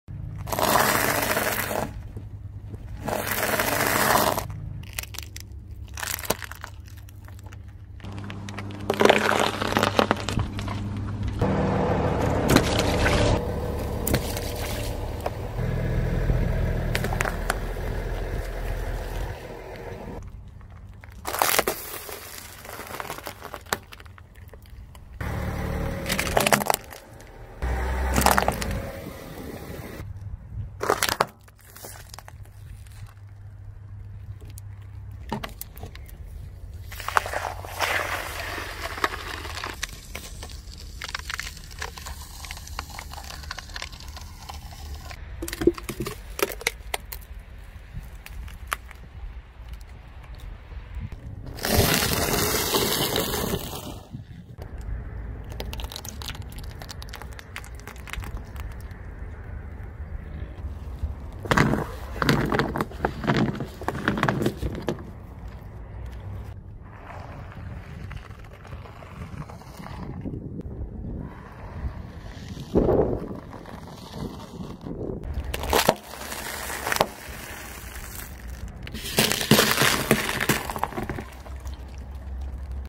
Experiment Car vs Lizard (toy) | Crushing Crunchy & Soft Things by Car
🔴 IMPORTANT INFO
I create all of my own content. Videos and Audio are recorded out in the field by myself.